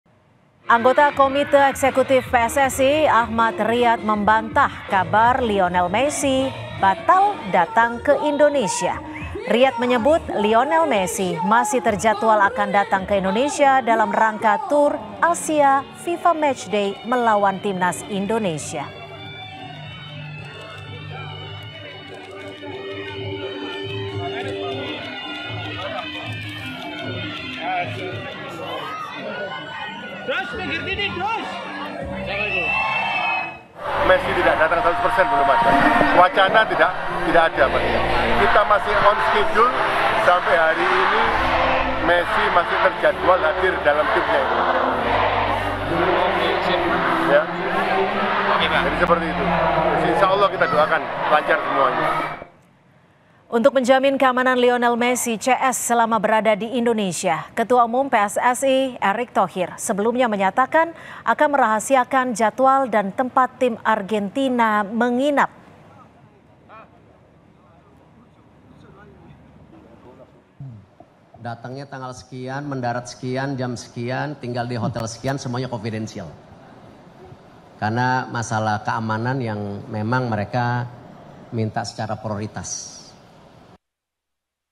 PSSI: Messi Masih Terjadwal Datang di FIFA Matchday Lawan Timnas Indonesia
KOMPAS.TV- PSSI: Messi Masih Terjadwal Datang di FIFA Matchday Lawan Timnas Indonesia